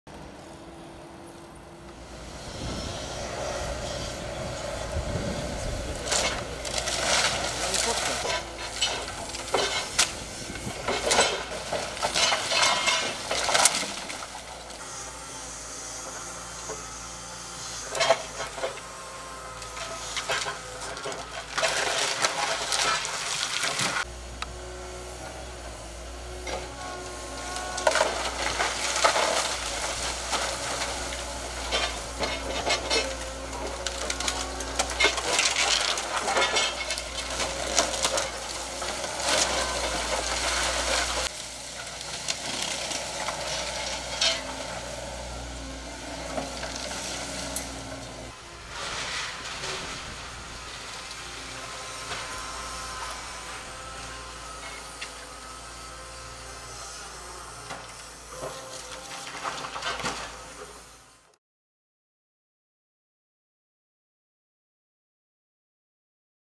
Demolice objektu v Honkově ulici
Dnes byla zahájena demolice objektu, který nahradí nová odlehčovací služba Honkova. Hotovo by mělo být za 18 měsíců.